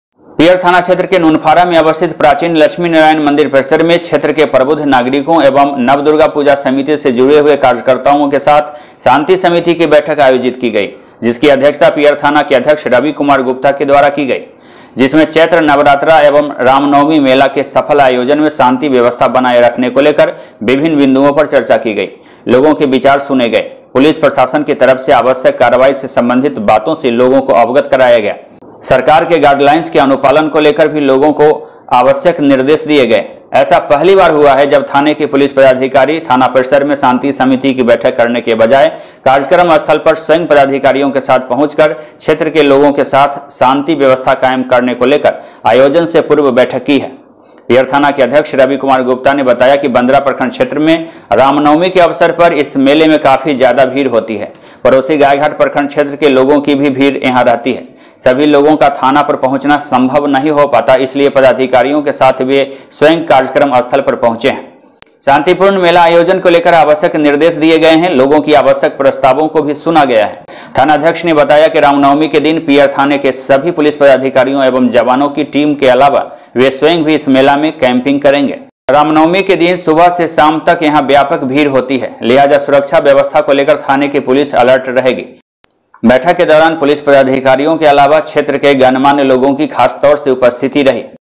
नुनफारा में रामनवमी मेला को लेकर पियर पुलिस ने की शांति समिति की विशेष बैठक#a2znewslive
नुनफारा में रामनवमी मेला को लेकर पियर पुलिस ने की शांति समिति की विशेष बैठक

दीपक कुमार तिवारी। बन्दरा।

पियर थाना क्षेत्र के नुनफारा में अवस्थित प्राचीन लक्ष्मी नारायण मंदिर परिसर में क्षेत्र के प्रबुद्ध नागरिकों एवं नव दुर्गा पूजा समिति से जुड़े हुए कार्यकर्ताओं के साथ शांति समिति की बैठक आयोजित की गई। जिसकी अध्यक्षता पीयर थाना के अध्यक्ष रवि कुमार गुप्ता के द्वारा की गई। जिसमें चैत्र नवरात्रा एवं रामनवमी मेला के सफल आयोजन में शांति व्यवस्था बनाए रखने को लेकर विभिन्न बिंदुओं पर चर्चा की गई। लोगों के विचार सुने गए।पुलिस प्रशासन की तरफ से आवश्यक कार्रवाई से संबंधित बातों से लोगों को अवगत कराया गया। सरकार के गाइडलाइंस के अनुपालन को लेकर भी लोगों को आवश्यक निर्देश दिए गए।ऐसा पहली बार हुआ है जब थाने की पुलिस पदाधिकारी थाना परिसर में शांति समिति की बैठक करने के बजाय कार्यक्रम स्थल पर स्वयं पदाधिकारियों के साथ पहुंचकर क्षेत्र के लोगों के साथ शांति व्यवस्था कायम करने को लेकर आयोजन से पूर्व बैठक की है। पियर थाना के अध्यक्ष रवि कुमार गुप्ता ने बताया कि बन्दरा प्रखंड क्षेत्र में रामनवमी के अवसर पर इस मेले में काफी ज्यादा भीड़ होती है। पड़ोसी गायघाट प्रखण्ड क्षेत्र के लोगों की भी भीड़ रहती है। सभी लोगों का थाना पर पहुंचना संभव नहीं हो पाता, इसलिए पदाधिकारियों के साथ वे स्वयं कार्यक्रम स्थल पर पहुंचे हैं। शांतिपूर्ण मेला आयोजन को लेकर आवश्यक निर्देश दिए हैं और लोगों की आवश्यक प्रस्तावों को भी सुना गया है।थानाध्यक्ष ने बताया कि रामनवमी के दिन पियर थाने के सभी पुलिस पदाधिकारियों एवं जबानों की टीम के अलावा वे स्वयं भी इस मेला में कैंपिंग करेंगे। रामनवमी के दिन सुबह से शाम तक यहां व्यापक होती है। लिहाजा सुरक्षा व्यवस्था को लेकर थाने की पुलिस अलर्ट रहेगी। बैठक के दौरान पुअनि एबी झा के अलावे स्थानीय सुंदेश्वर राम, रत्नेश झा, अजय राय, पंकज कुमार, विजय झा, हरिनाम झा, राकेश कुमार, उदय कुमार साह,विजय झा, सोनू कुमार, मदन ठाकुर,राजीव कुमार, राकेश कुमार ठाकुर, सुनील कुमार ठाकुर आदि भी मौजूद थे।